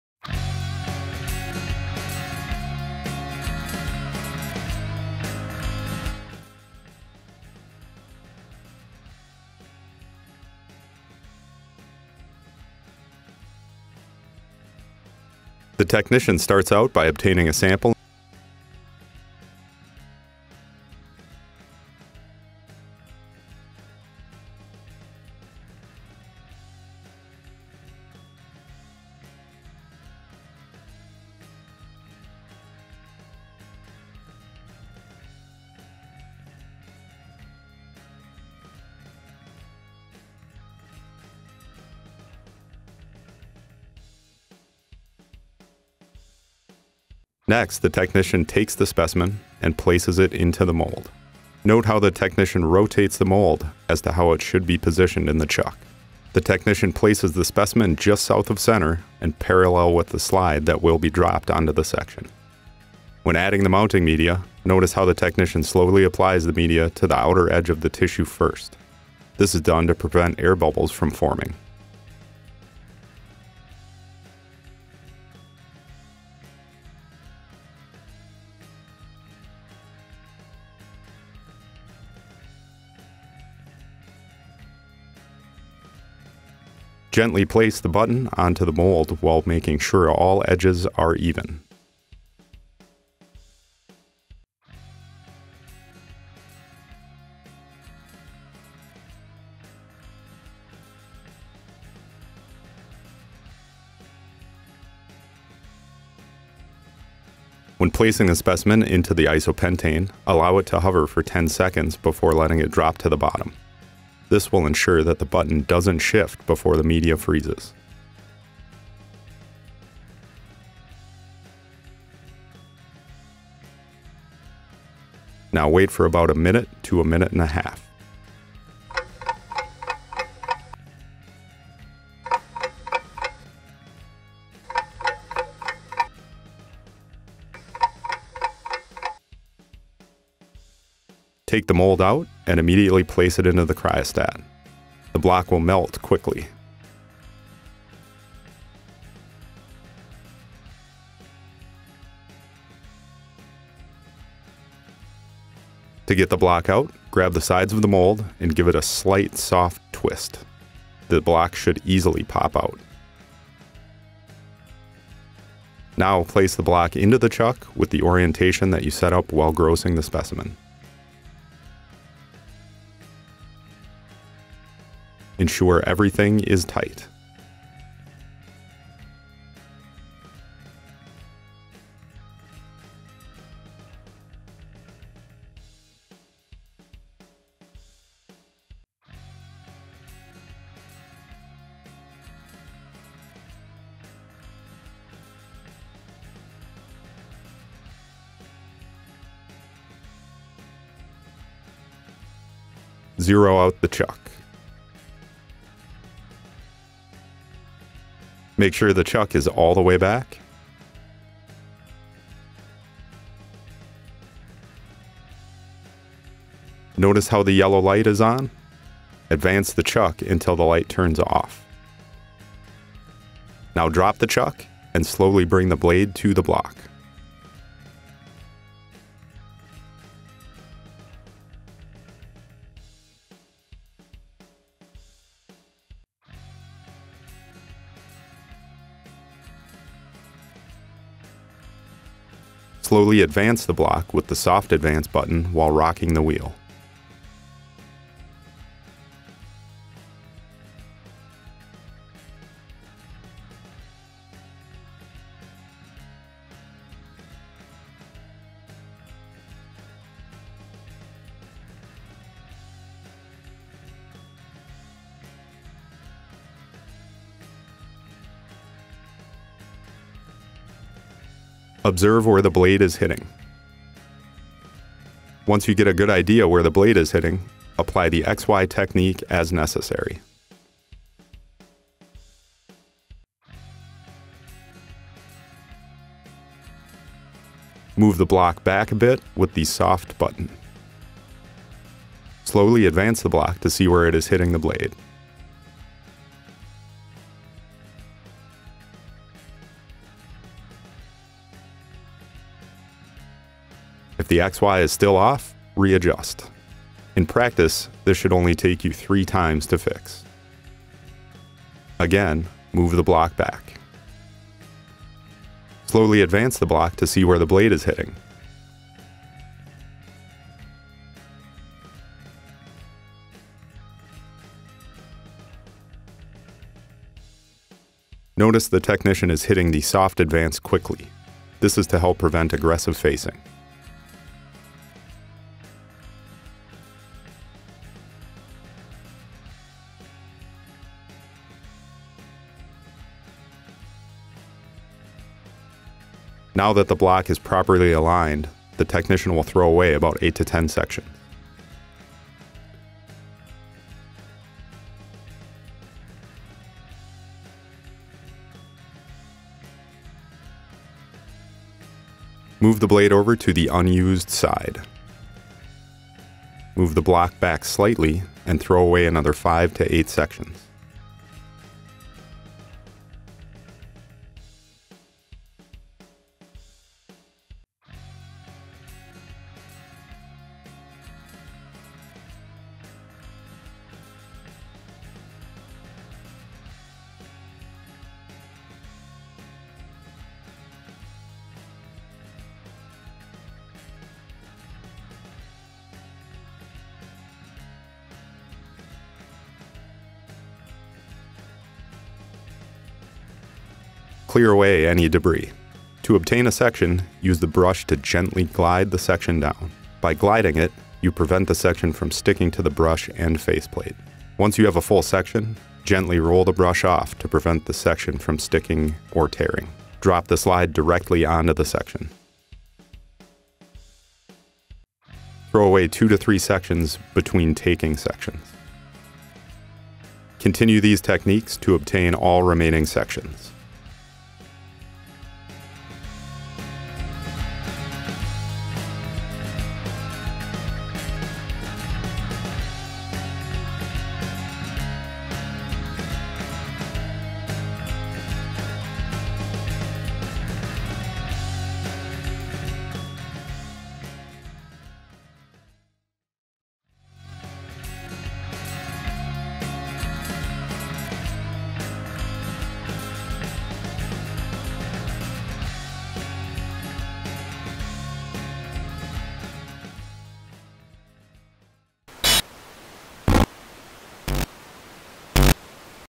Cryotomy Basics
Introduction to the basics in cutting sections on a cryostat.